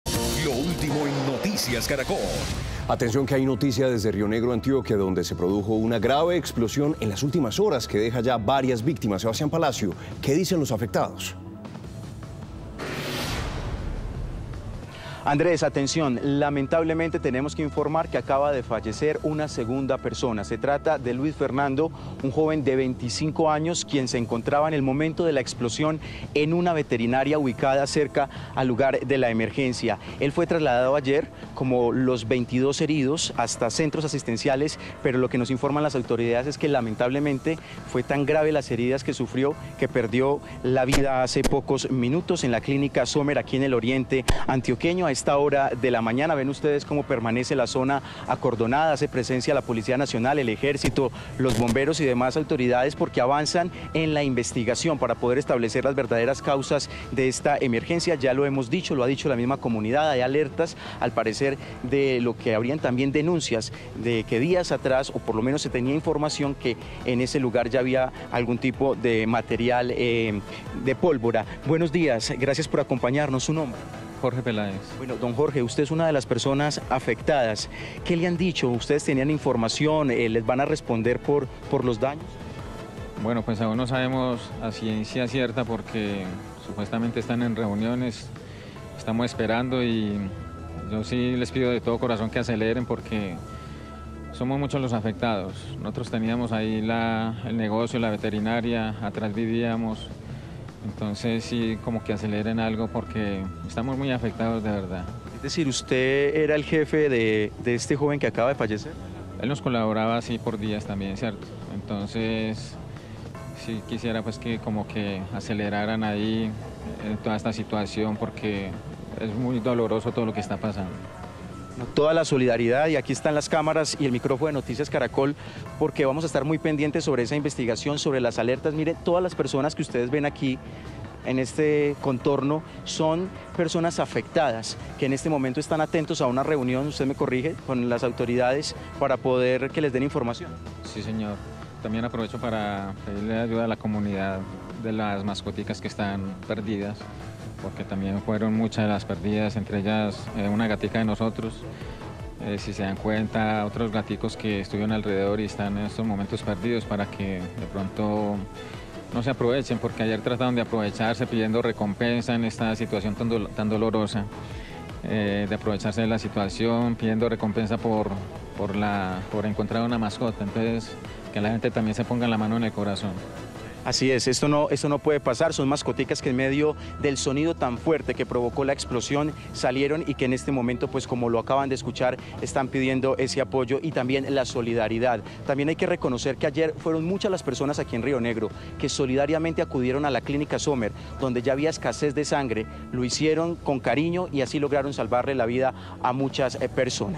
Se elevan a dos los muertos por explosión en Rionegro, Antioquia
Las autoridades informaron que un joven de 25 años, que había sido trasladado a la Clínica Somer, falleció por las heridas que sufrió a causa de la explosión en Rionegro, Antioquia, en una casa que, según las denuncias, funcionaba como polvorería ilegal.